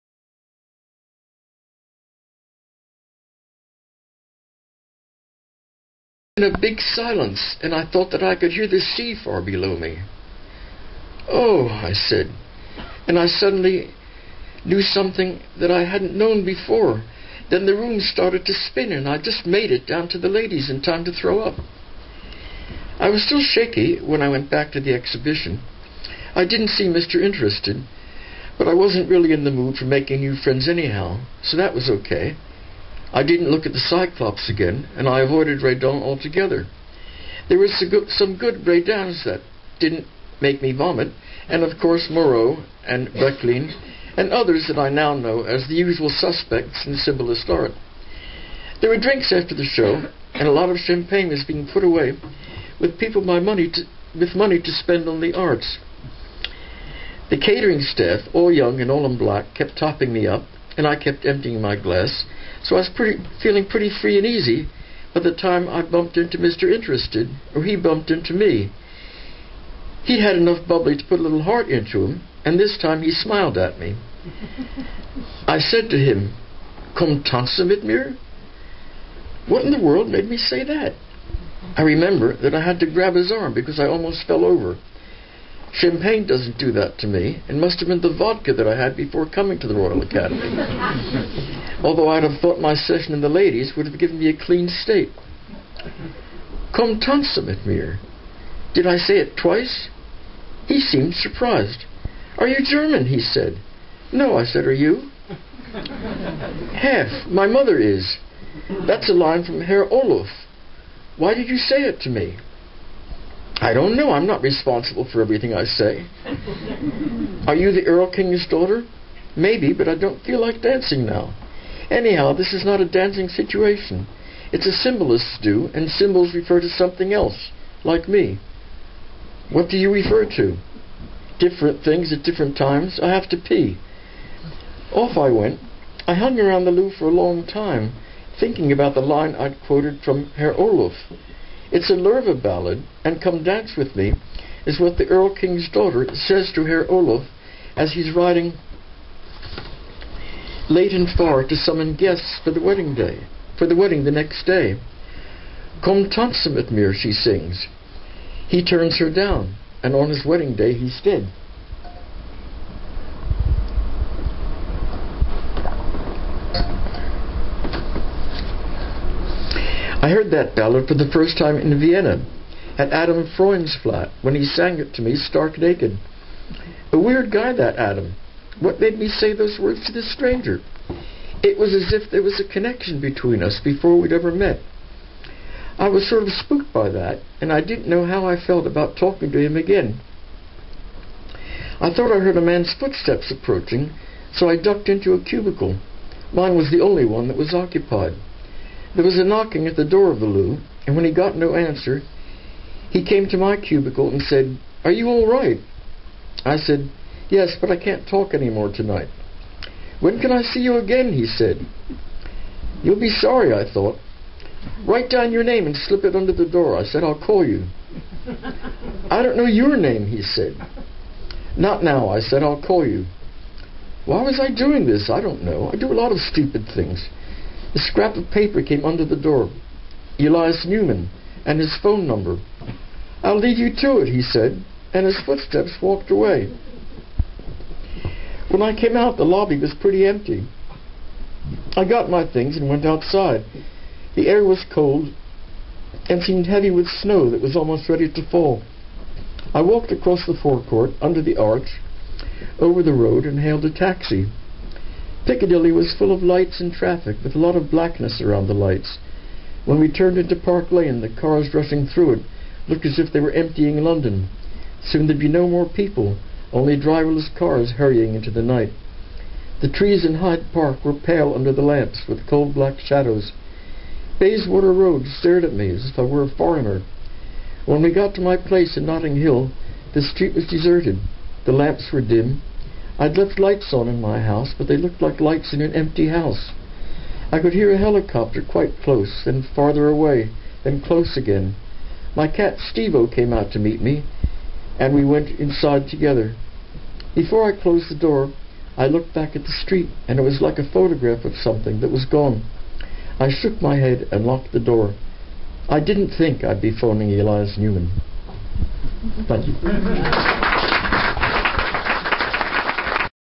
Russell Hoban reading from 'Come dance with me'
Russell Hoban reading from 'Come dance with me, recorded at the Russell Hoban Some-Poasyum,
London, UK, 11-13 February 2005